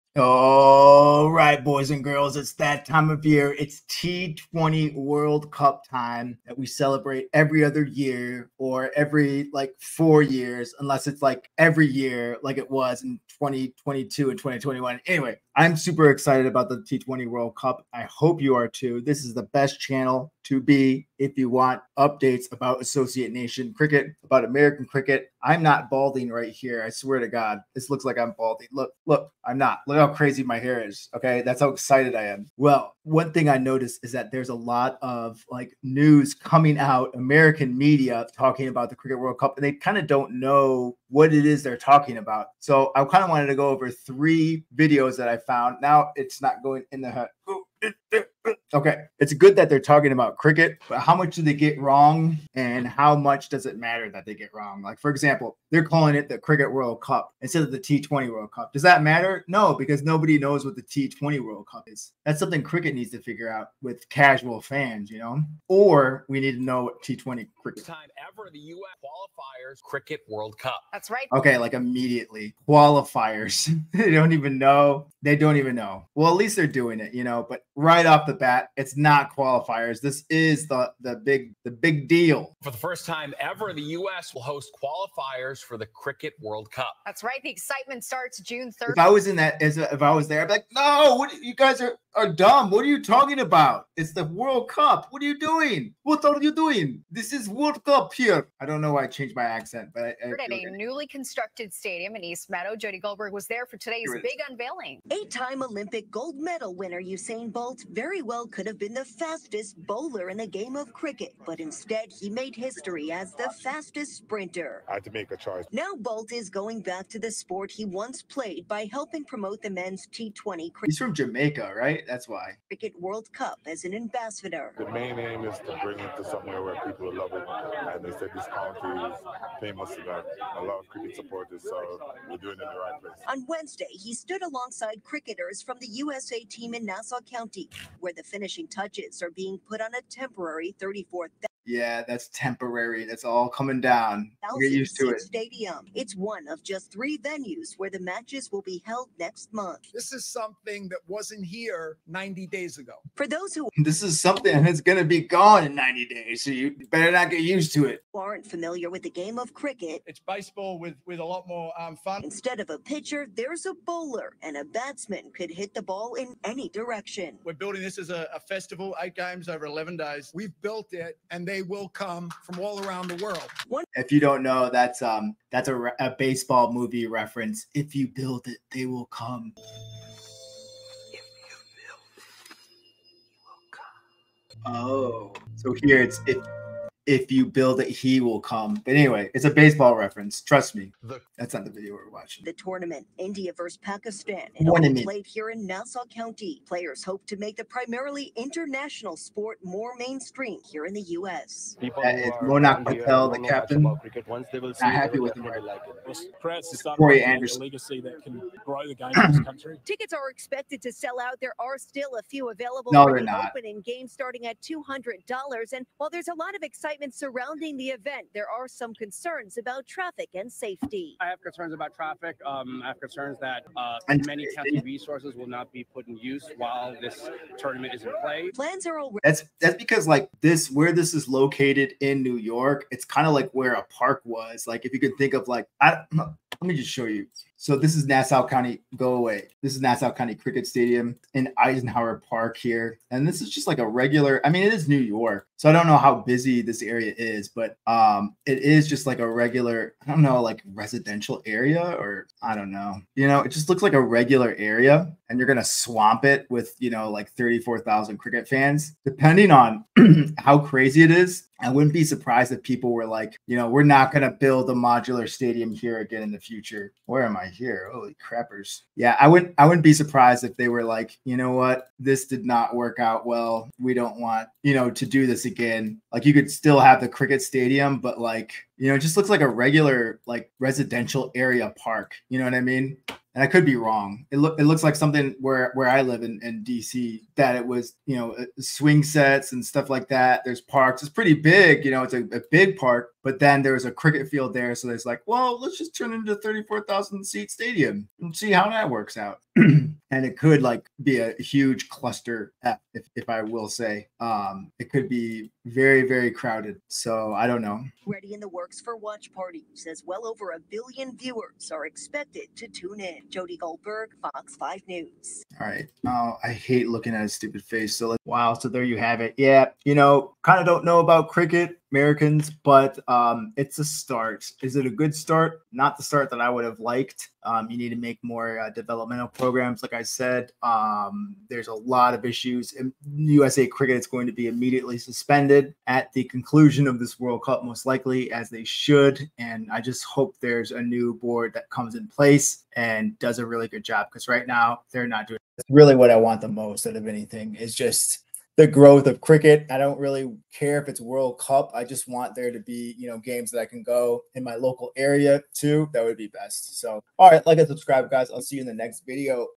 What is American Media Saying About the T20 Cricket World Cup?
BEST SOURCES FOR USA CRICKET

^ I'm unaffiliated with the above but they are best source for USA Cricket and infinitely better and more knowledgeable than I am.